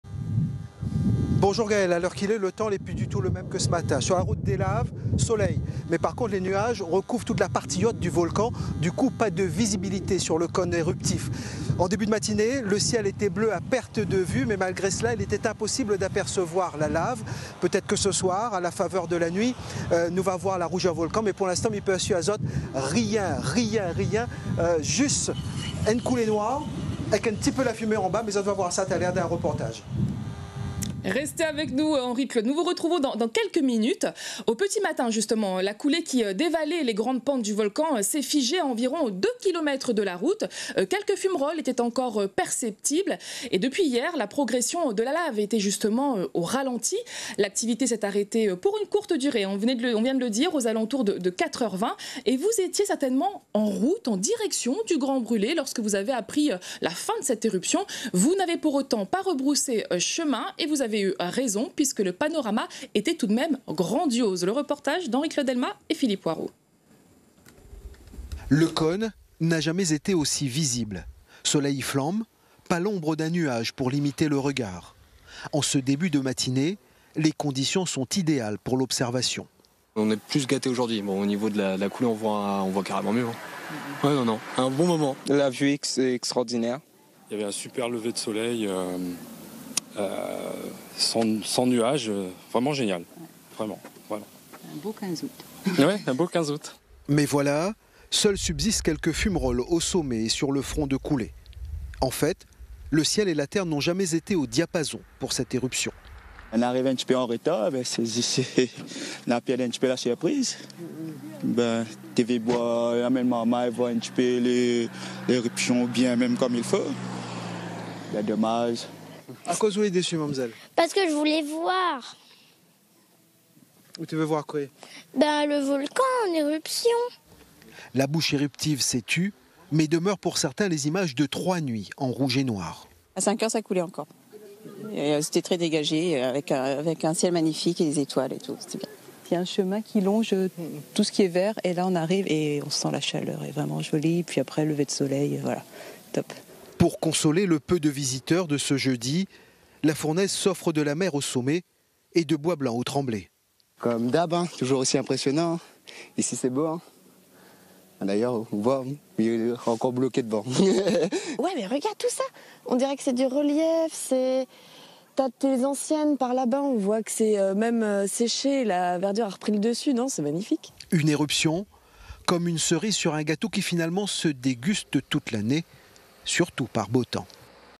La reprise de l'éruption du Piton de la Fournaise
Après une pause de 4h, l’éruption du Piton de la Fournaise a repris ce jeudi à 8h30 au même endroit. Est-elle toujours visible depuis la route des Laves ? Le point sur place avec Réunion La 1ère :